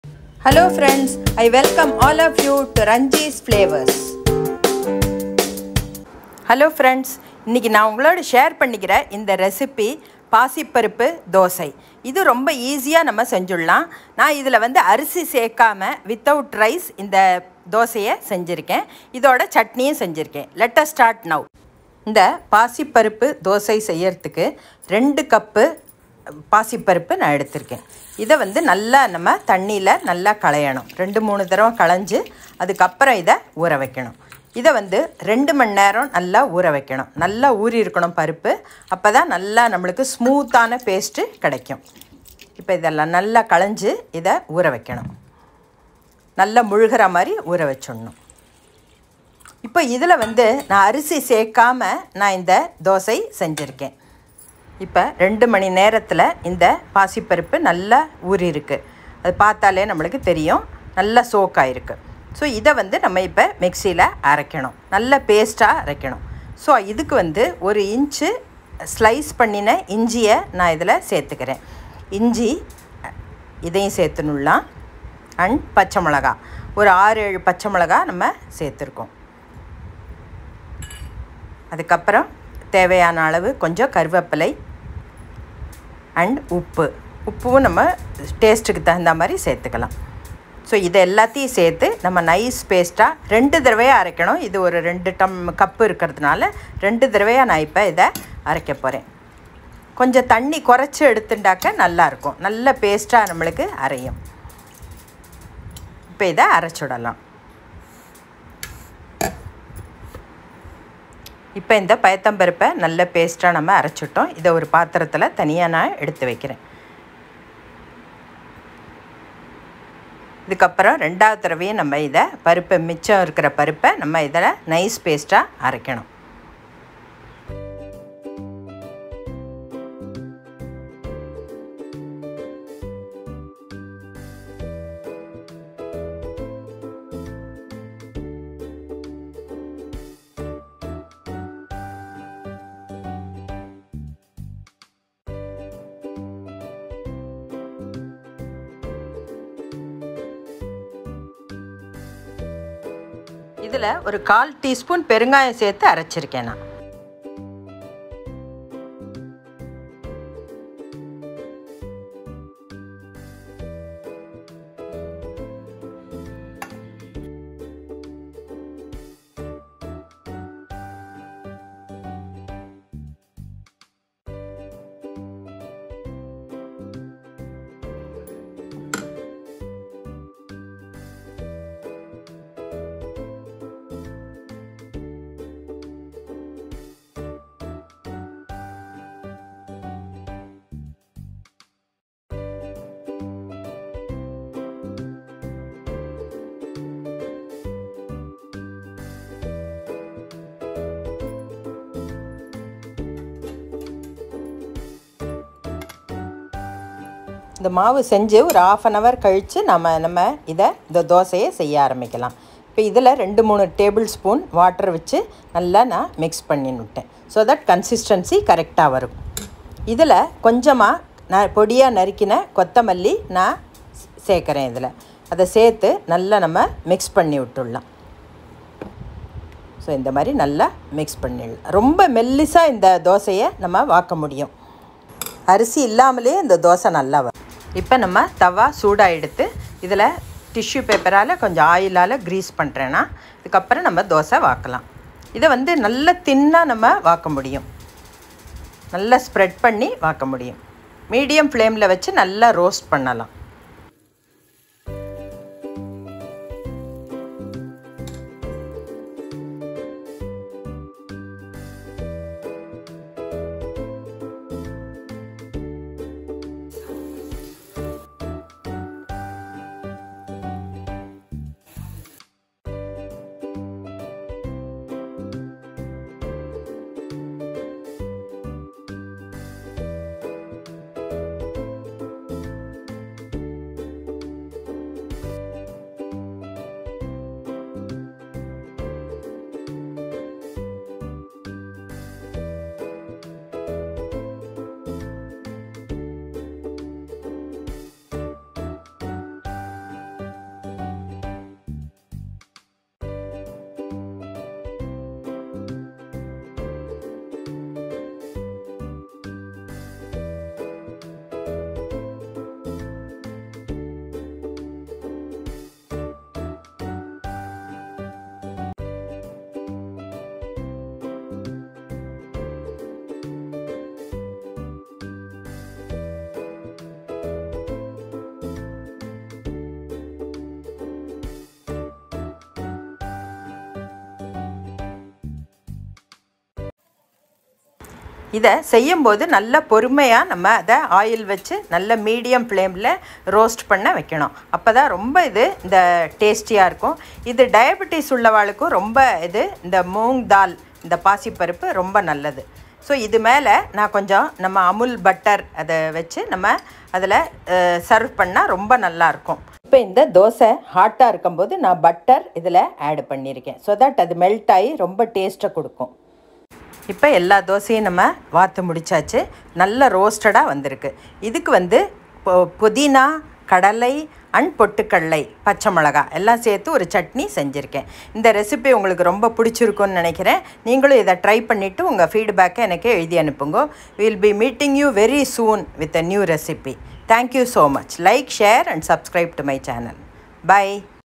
Pasi Paruppu Dosai/Instant High protein Moong Dal Dosa Recipe/How to make Mung Dal Dosa without Rice
Ingredients:

பாசிப்பருப்பு / Green gram lentil- 2 cups

இஞ்சி / Ginger- 1 inch sliced

பச்சை மிளகாய் / Green chili- 6 to 7

பெருங்காயத்தூள்/ Asafetida- 1/4 tsp

கருவேப்பிலை / Curry leaves- little

கொத்தமல்லித்தழை / Coriander leaves- little

எண்ணை / Oil- to make dosa

Method:

Wash the mung dal thrice & soak in water for 2 hrs.

After 2 hrs grind the dal with green chili, ginger, salt , curry leaves & Asafetida to a fine paste.

Rest the batter for 30 minutes. Heat Dosa Tawa.

Make dosa in a thin form & pour oil around & in a medium flame. Flip to the other side and let it get roasted. Place butter on top of Dosa & serve hot with yummy chutney.

Make chutney with mint, groundnut, roasted Bengal gram, green chili & salt.
Enjoy Ranjis Flavours!